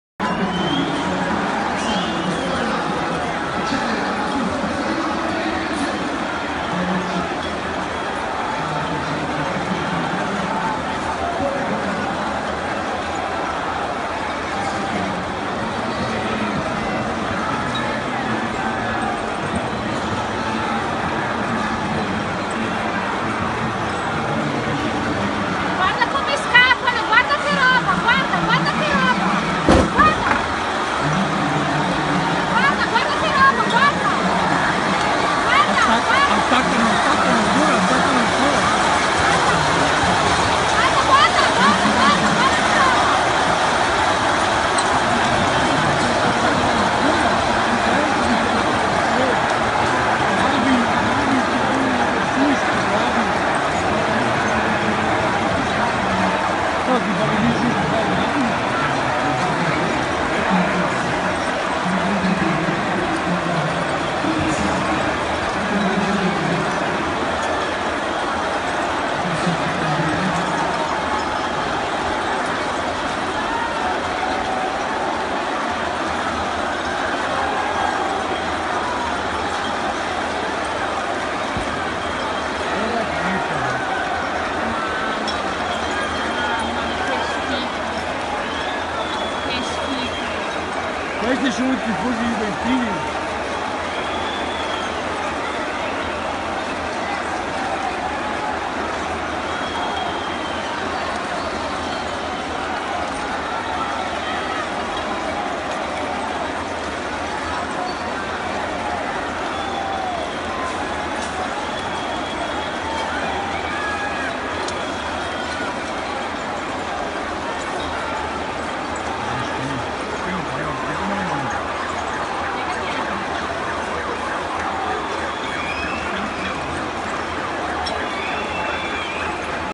Paura Attentato in Piazza San Carlo a Torino 3 Giugno 2017 Juve Real Madrid
Paura attentato, Panico feriti anche gravi e gran caos a Torino in piazza San Carlo Durante la finale di Champions forse per colpa di un petardo lanciato scatenando cio che vedete nelle immagini.
Video prelevato fa Facebook